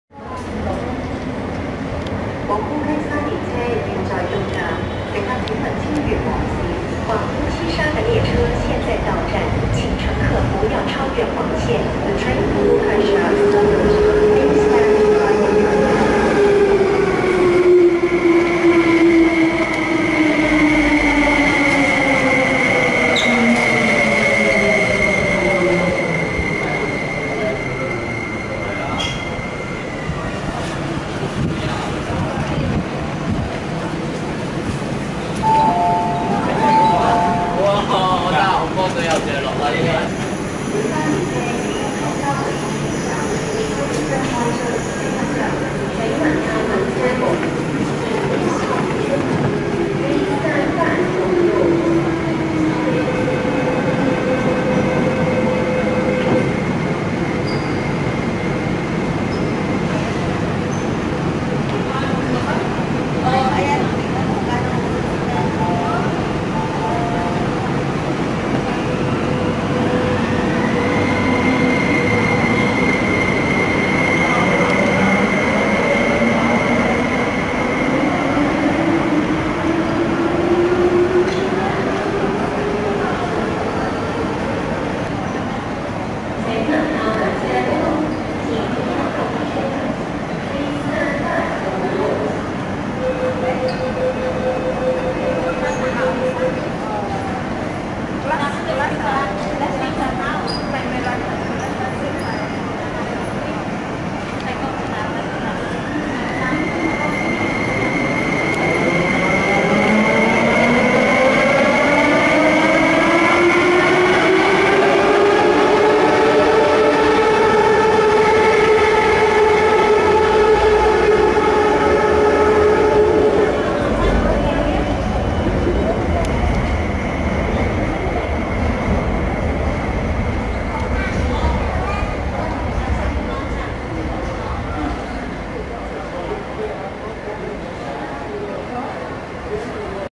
【馬鐵新開始】港鐵馬鞍山線大圍站 (統一開關門提示聲) MTR Ma On Shan Line Tai Wai Station
還是原本的廣播較好...